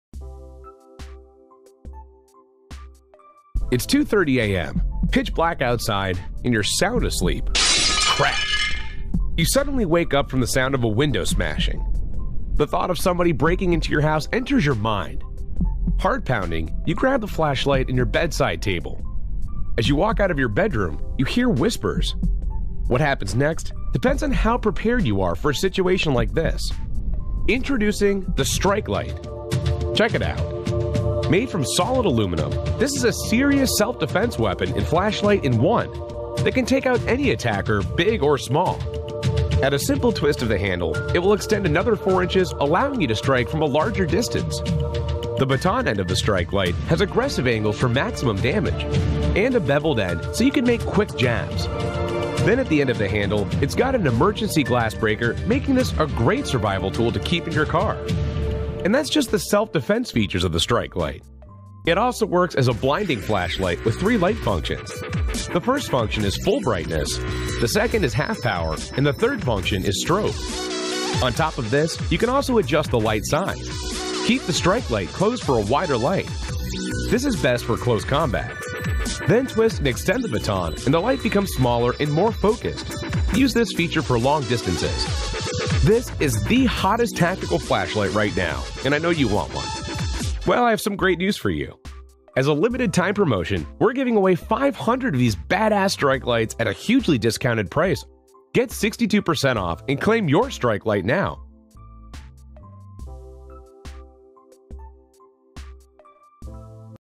Ape Survival StrikeLight Torch | Best Self-Defense Tactical Flashlight | Claim Now For FREE
Hi Guys... This video is about "Ape Survival StrikeLight Torch | Best Self-Defense Tactical Flashlight | Claim Now For FREE"

Blinding torch can drop an attacker like a sac of potatoes!

Made from solid aluminium, this is a serious self-defense tool that can take out any attacker, big or small.

At simple twist of the handle this will extend another 4 inches allowing you to strike from a large distance.

It also works as a blinding flashlight with 3 light functions.

The first function is full brightness, the 2nd is half power and the 3rd function is strobe lights.

On top of this you can also adjust the light size. Keep the strike light closed for a wider light.

This is best for close combat. Then twist and extend the baton and the light becomes smaller and more focused. Use this feature for long distances.